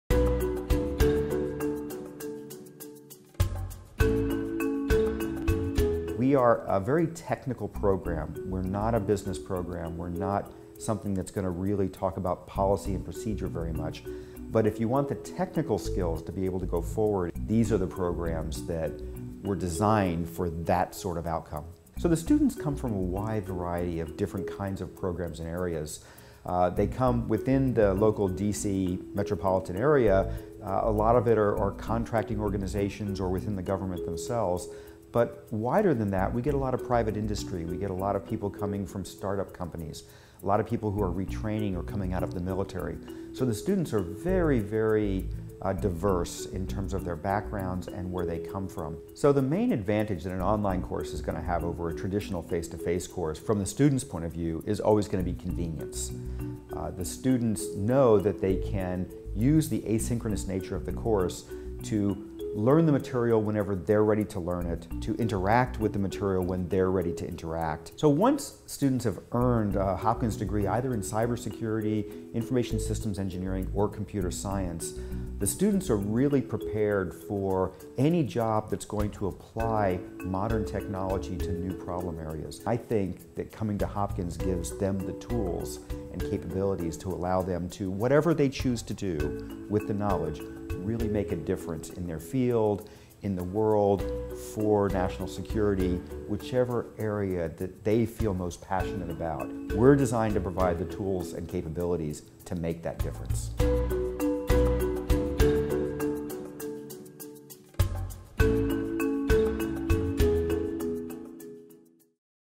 Computer Science, Cybersecurity, and Information Systems Engineering: Message from the Program Chair
Program Chair Tom Longstaff gives us an overview of the part-time Computer Science, Cybersecurity, and Information Systems Engineering programs at Johns Hopkins Engineering.